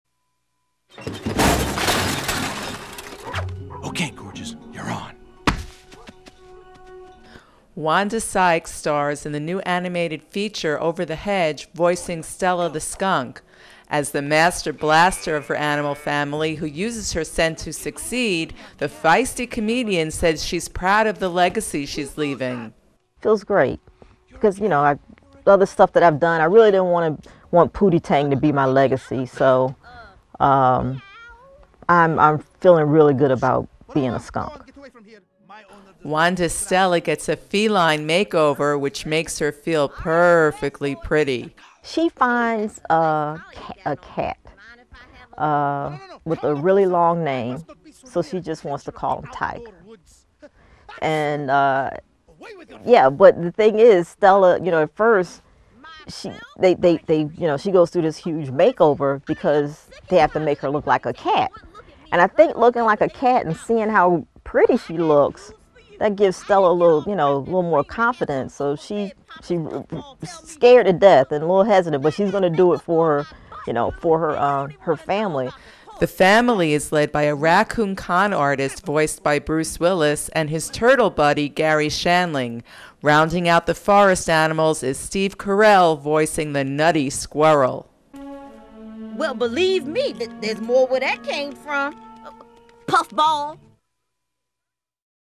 WANDA SYKES PSYCHED ABOUT STELLA FOR OVER THE HEDGE
Wanda Sykes stars in the new animated feature Over The Hedge voicing Stella the skunk. As the master blaster of her animal family who uses her scent to succeed, the feisty comedian says she's proud of the legacy she's leaving. "It was great because you know the other stuff that I've done I really didn't want Pooty Tang to be my legacy. So I'm feeling really good about being a skunk." Wanda's Stella gets a feline make-over which makes her feel perfectly pretty. "She's finds a cat with a really long name so she just wants to call him Tiger. The thing is Stella at first goes through this huge make-over because they have to make her look like a cat. I think looking like a cat and seeing...